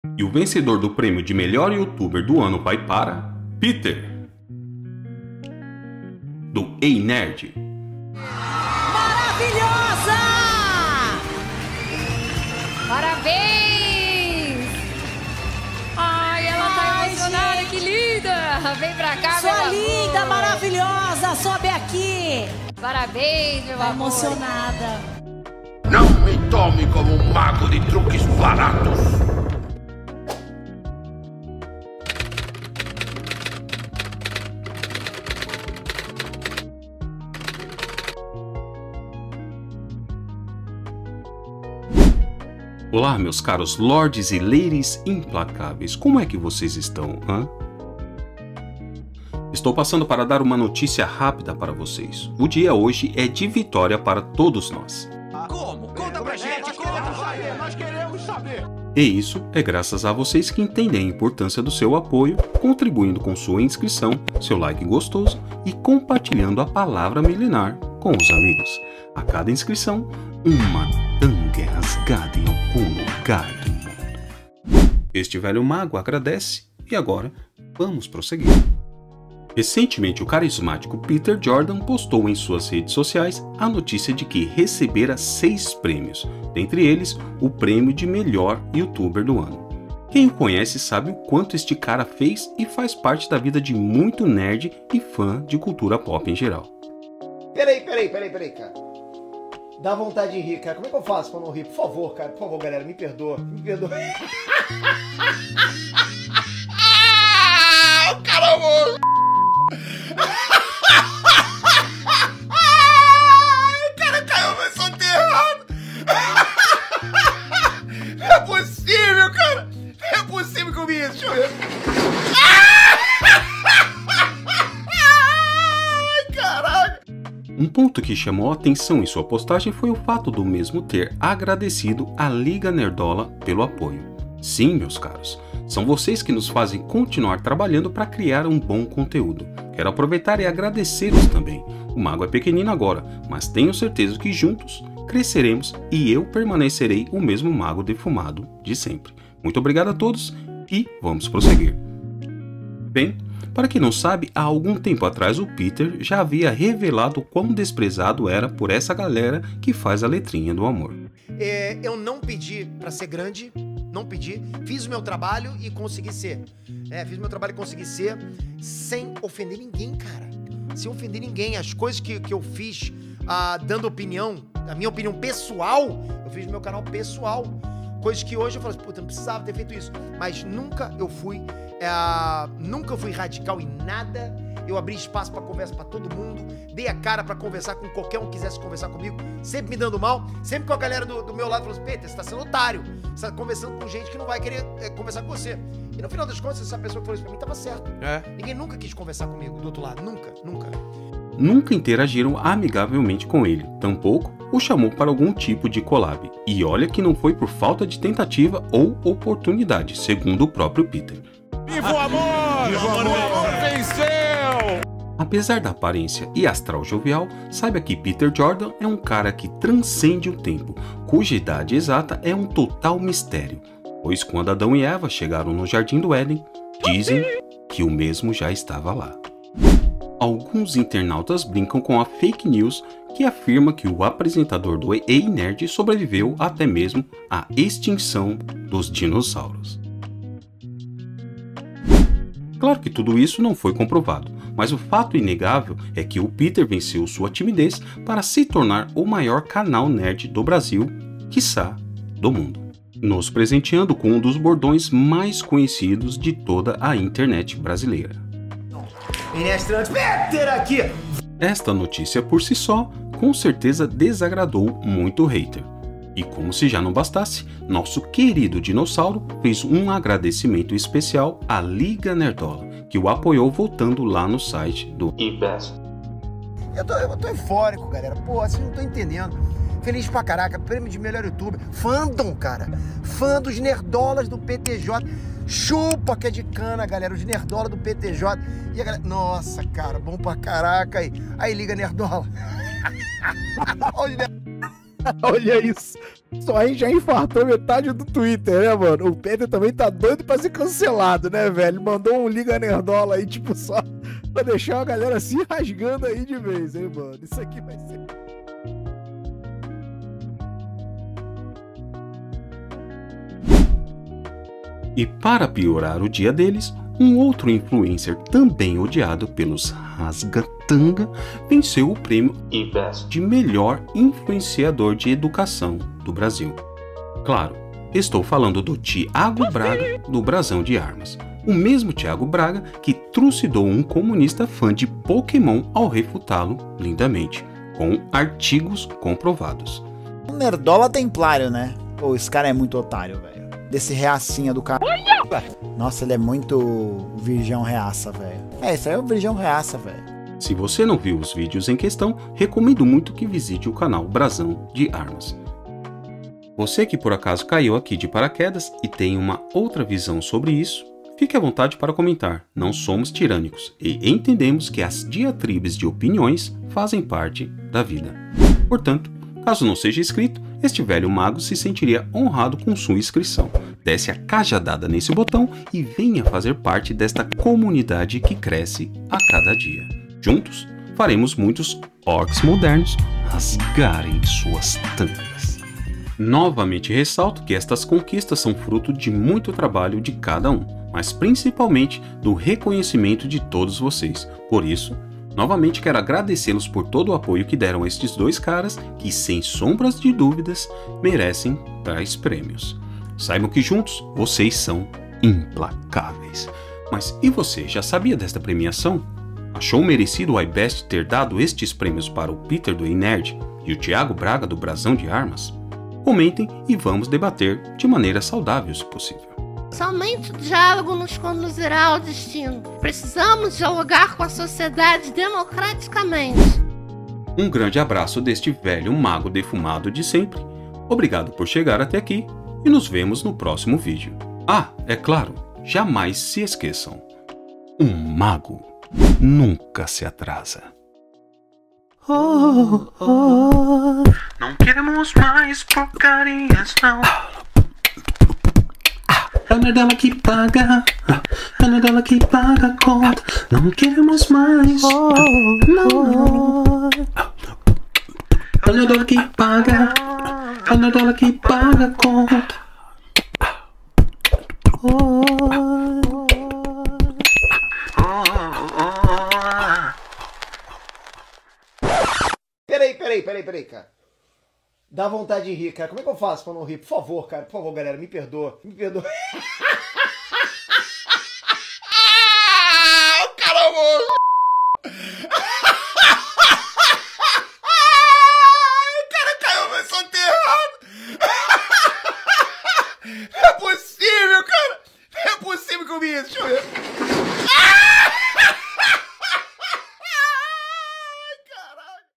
PETER EI NERD E THIAGO BRAGA VENCEM PRÊMIO IBEST | AGRADECIMENTOS, NERDOLAS E MUITA RISADA
O TWITTER esrava euforico com a postagem do dinossauro Peter Jordan, que levou para casa nada mais nada menos que 6 prêmios.

Thiago Braga do Brasao de Armas levou prêmio de melhor influencer na area da educação.

Com certeza tem gente que pode não admitir, mas o cotovelo deve estar tininduuuum até agora. rsss

CONFIRA TAMBÉM.